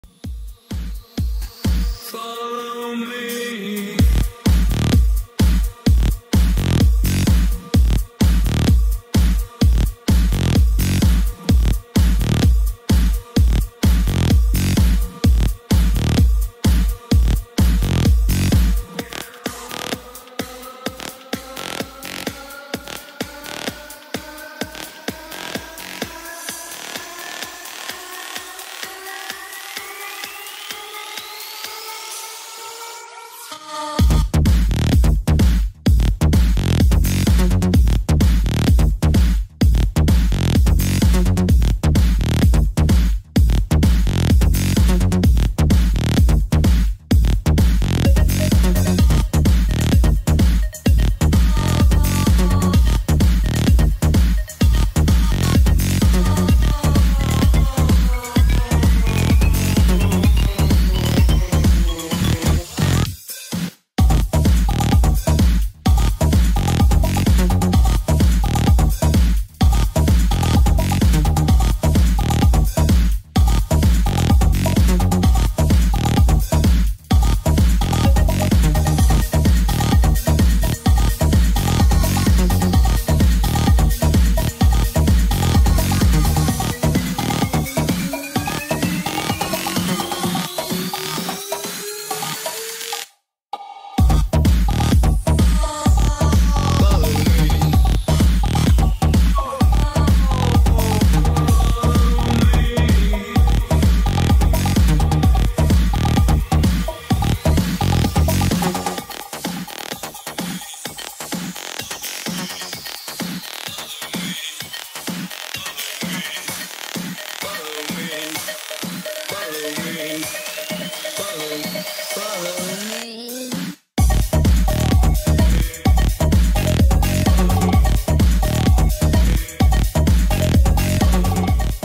Raver's Fight Night by Melodic Nights Agency [13-Nov-2014]
Another great event in Cairo with DJ Sanka from Brazil and DJ lu4o from Bulgaria , along with the best underground DJs inside Egypt ,